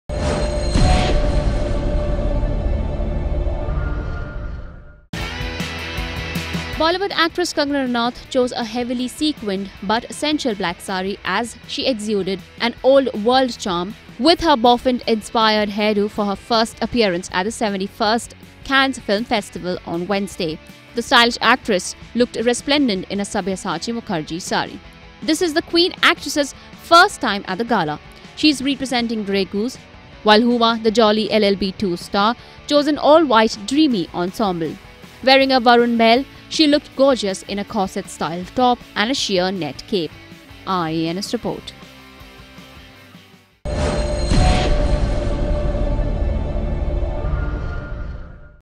Black & White Kangana & Huma at Cannes
Bollywood actress Kangana Ranaut chose a heavily sequinned but sensual black sari as she exuded an old world charm with her bouffant-inspired hairdo for her first appearance at the 71st Cannes Film Festival on Wednesday. While Huma the 'Jolly LLB 2' star chose an all white dreamy ensemble. Wearing a Varun Bahl she looked gorgeous in a corset styled top and a sheer net cape.